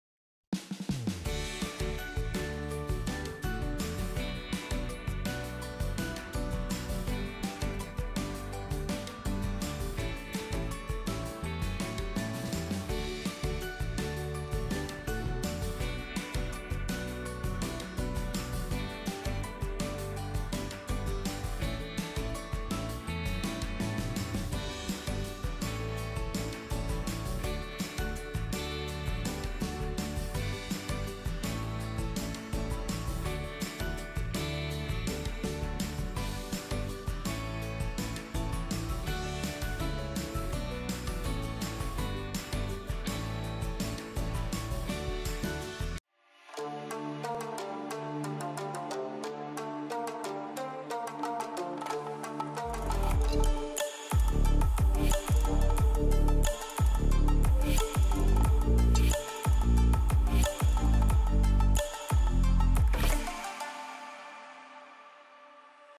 How to find verification code hilook dvr | hilook stream encryption
This video explain how to find verification code hilook dvr | hilook stream encryption. This Video also related to hilook dvr verification code, hilook cctv encryption password, hilook verification code, hik-connect verification code, hilook input verification code, and hilook encryption password.